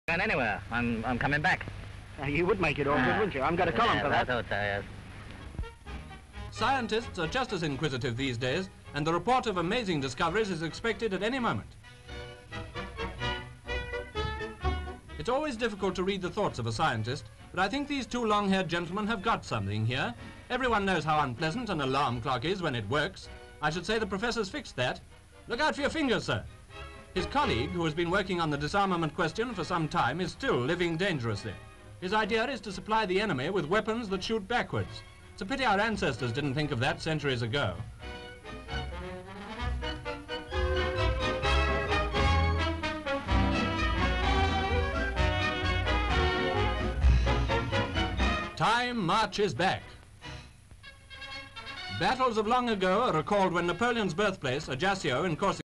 Monkey "Engineers"
Monkeys are seen fooling around with an alarm clock and a gun. They attack these objects, armed with a hammer and chisel and a brace and bit respectively. Then one of them smokes a well-earned cigarette.